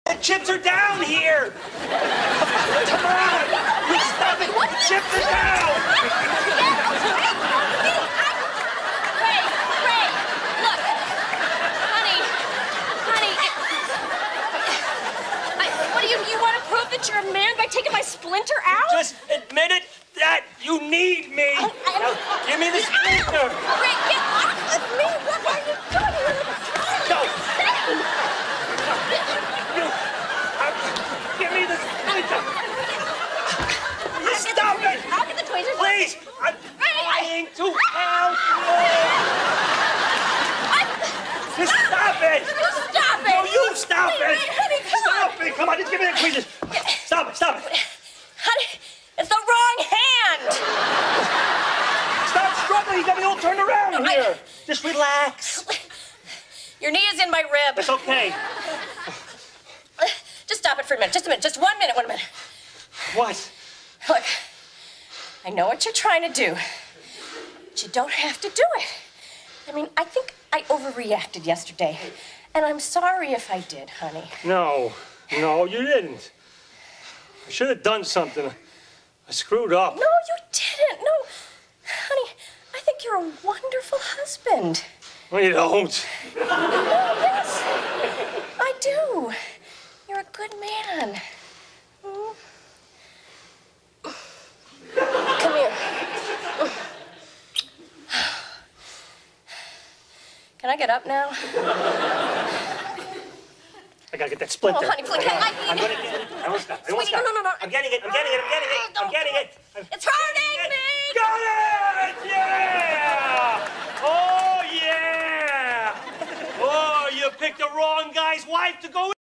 ray playfighting with debra
ray is trying to prove he is a man by trying ot get a spltiner out of debra's hand